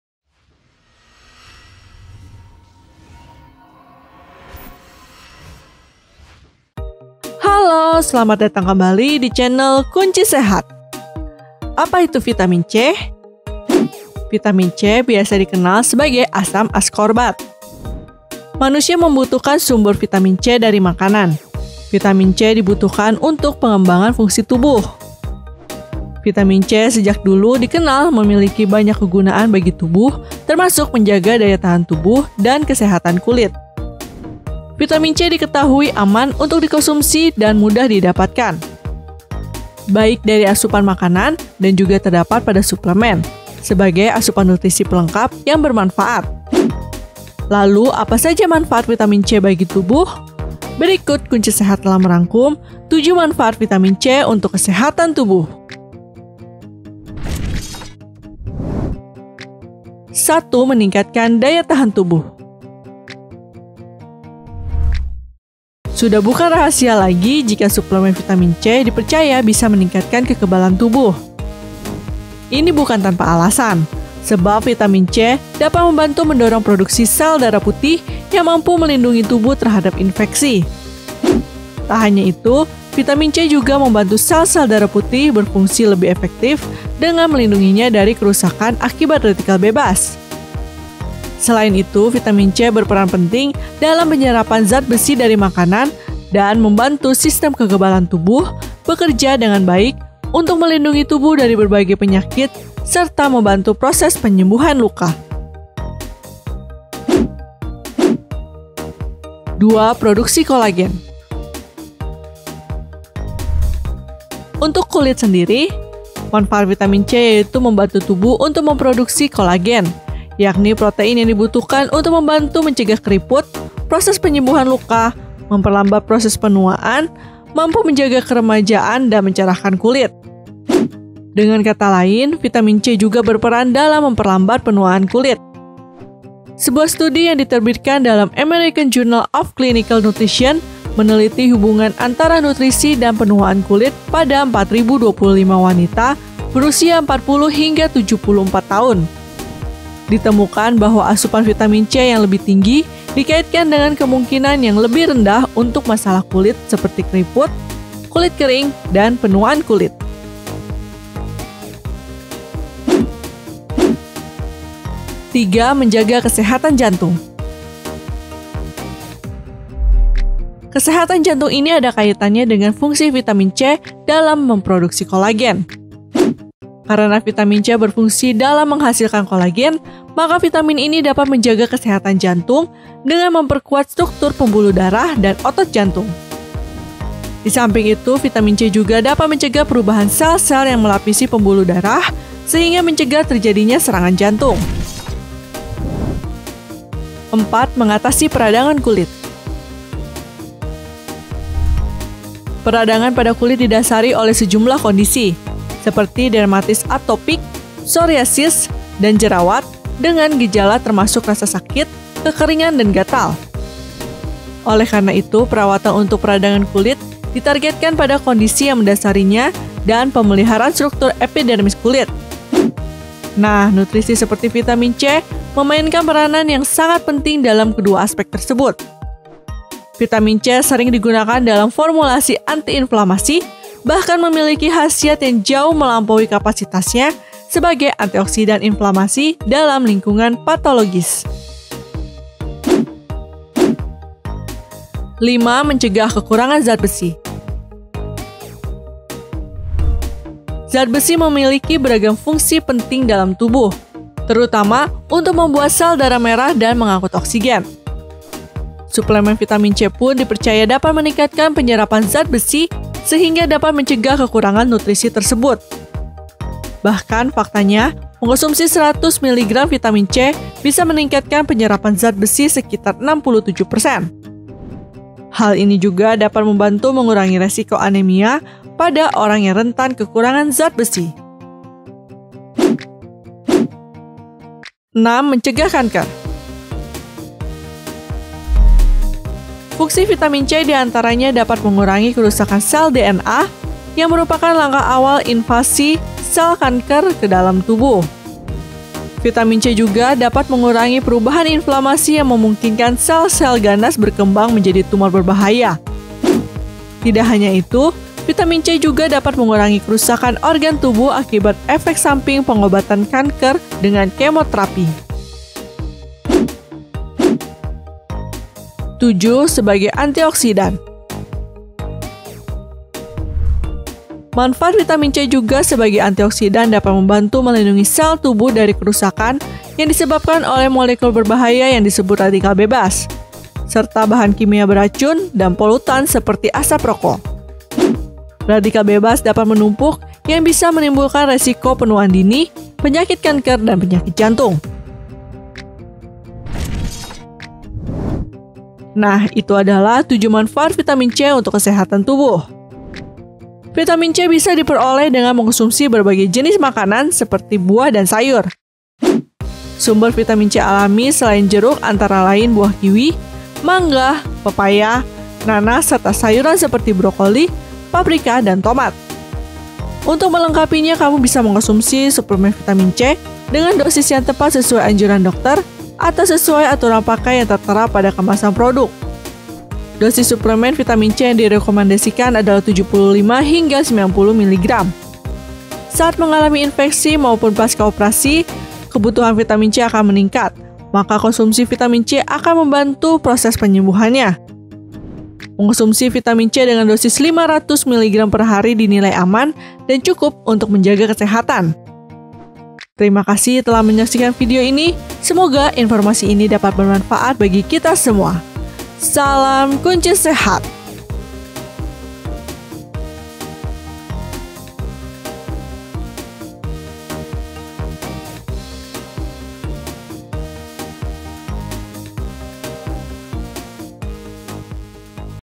7 Manfaat Vitamin C untuk kesehatan tubuh
Siapa yang suka dengan vitamin C? Biasa vitamin c terdapat dalam buah buahan terutama buah jeruk. Vitamin C biasa dikenal sebagai asam askorbat. Manusia membutuhkan sumber vitamin C dari makanan.  Vitamin C dibutuhkan untuk pengembangan fungsi tubuh.
Vitamin C sejak dulu dikenal memiliki banyak kegunaan bagi tubuh, termasuk menjaga daya tahan tubuh  dan kesehatan kulit. Vitamin C diketahui aman untuk dikonsumsi dan mudah didapatkan. Baik dari asupan makanan  dan juga terdapat pada suplemen, sebagai asupan nutrisi pelengkap yang bermanfaat. lalu apa saja manfaat vitamin c bagi tubuh,

Tonton terus videonya ya.
Terima kasih.